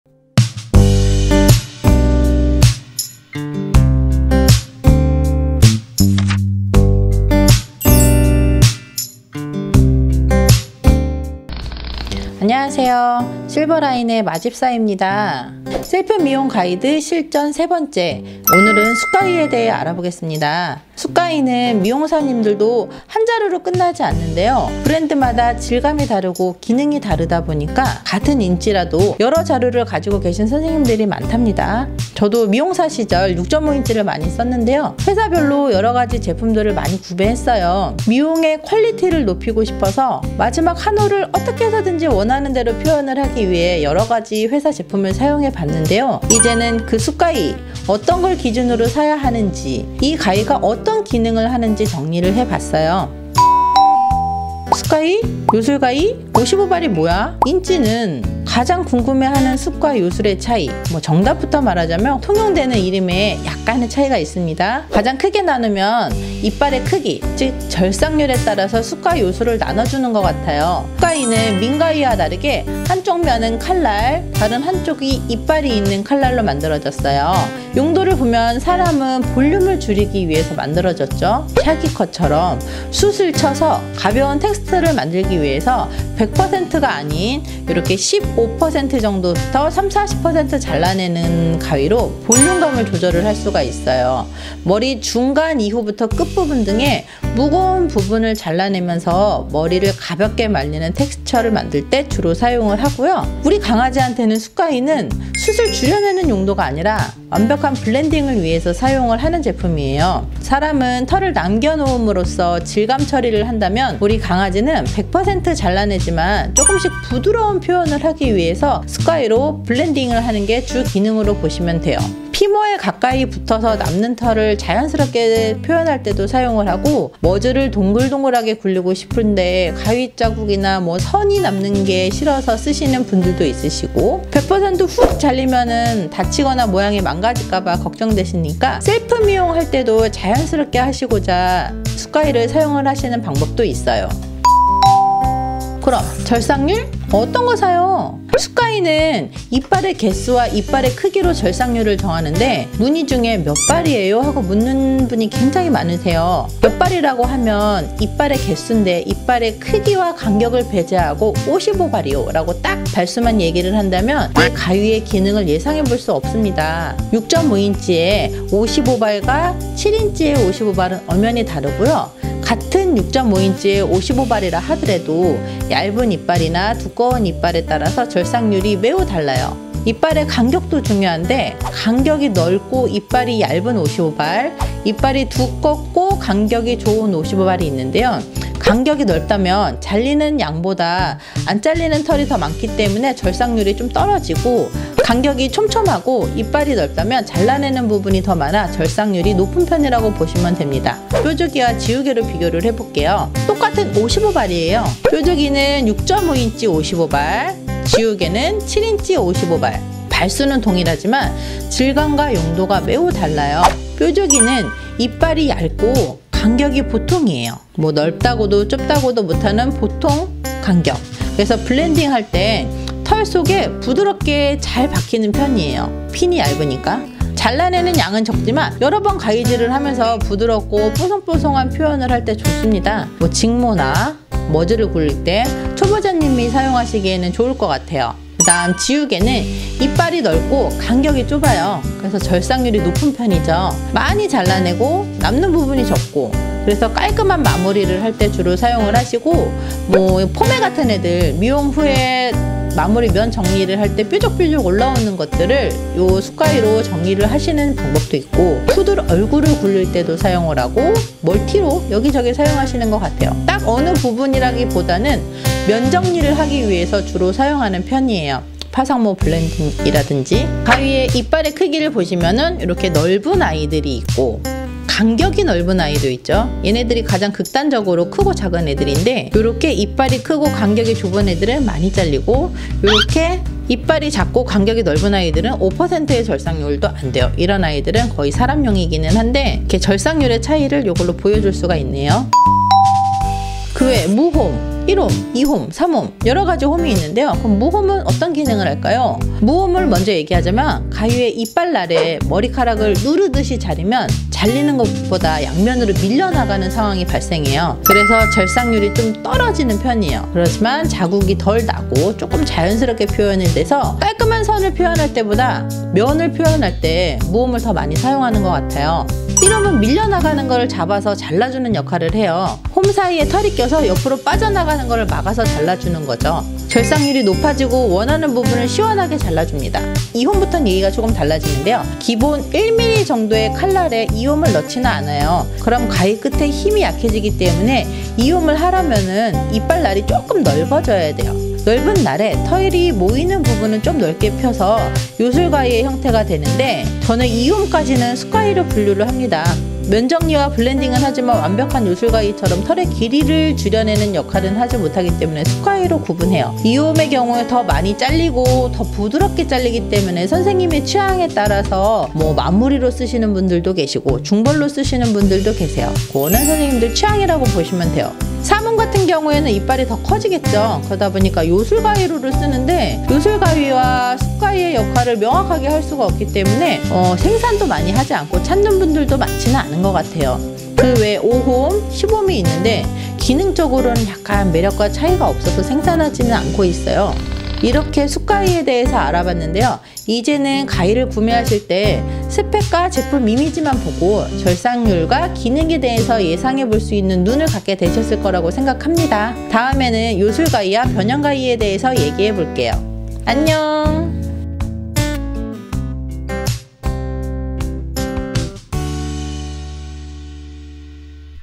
애견 셀프 미용 가이드 3편 - 절삭률? 홈? 숱가위는 이렇게 보자!
강아지 털의 자연스러운 블렌딩을 위해 숱가위는 필수!
그런데, 숱가위의 이빨에 따라 절삭률와 스타일이 달라집니다!
알고 구매하면 좋겠죠?

애견미용 업계 트렌드 리더가 되겠습니다!
일산본점 영업시간 평일 09:00~17:00